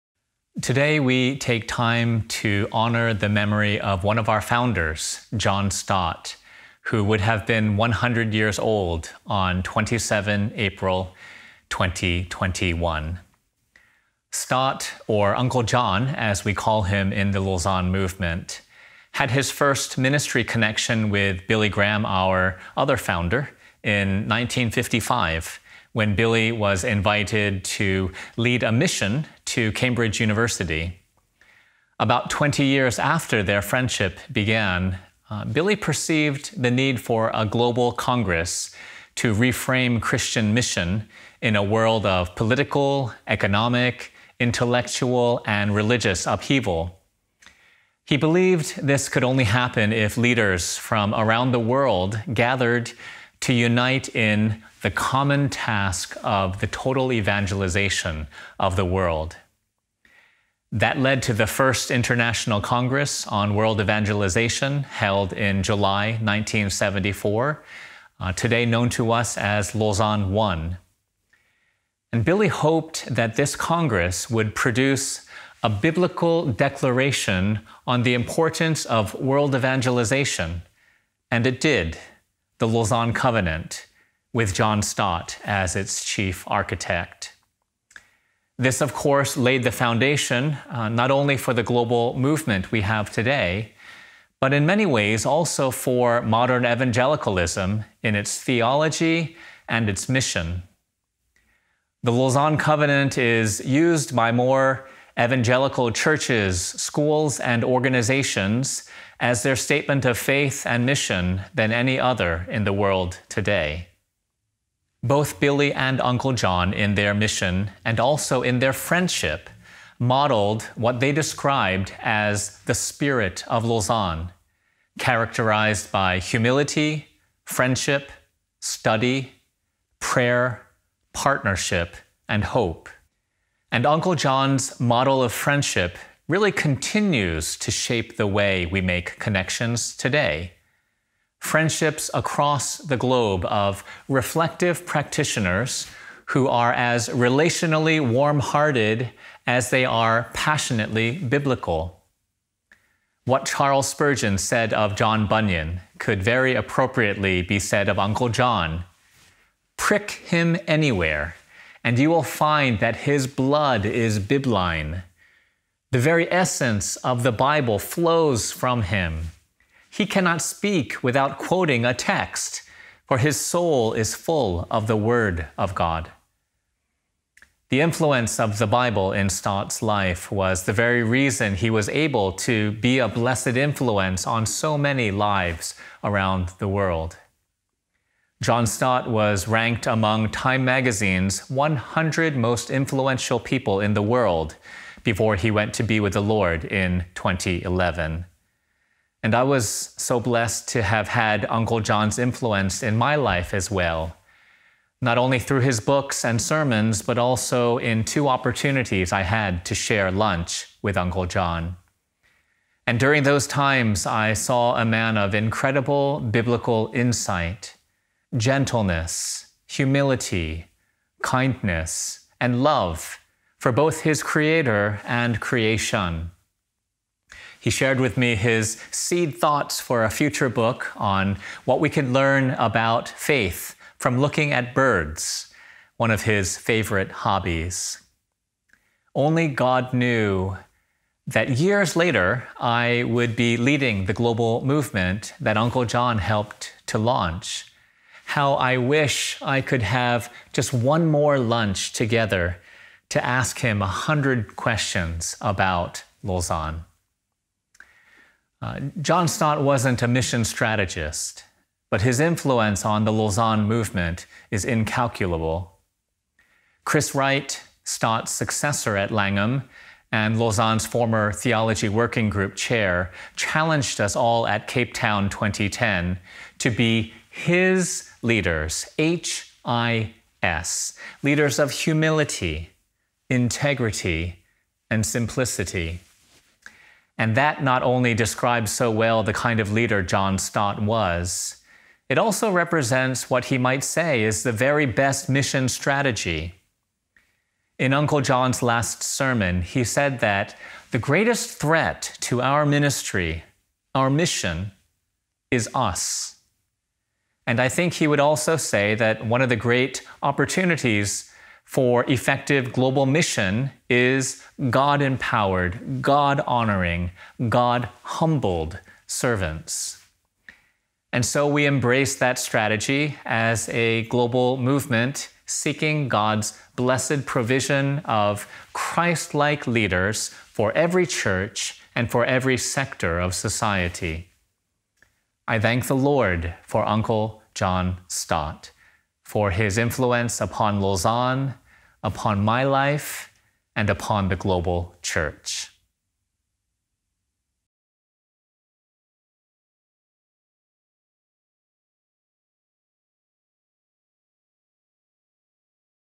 A Tribute to John Stott 100 Years After His Birth
Lausanne's global executive director and CEO Michael Oh shares a tribute to John Stott, the co-founder of the Lausanne Movement, marking the centenary of his birth on 27 April 1921. 'Uncle John' (as he is affectionately known in the Movement) played an instrumental role in bringing the global church together for the First Congress on World Evangelization in July 1974 and served as the chief architect of The Lausanne Covenant. He was involved in the formation and development of the Lausanne Movement beyond 1974 and held various positions within the Movement until his death in 2011.